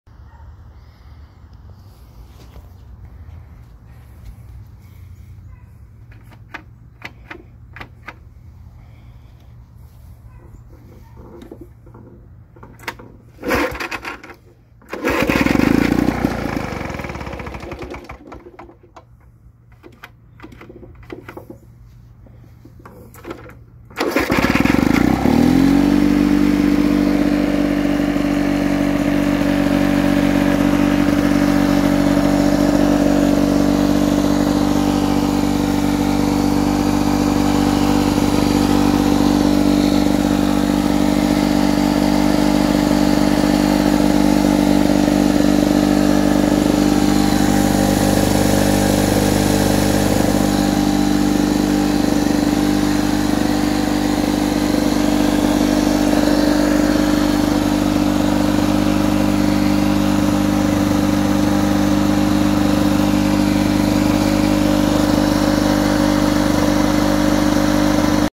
1967/1968 Lawn-Boy 3054 Start Up
This is a cold start of a 1967 or 1968 Lawn-Boy 3054 mower.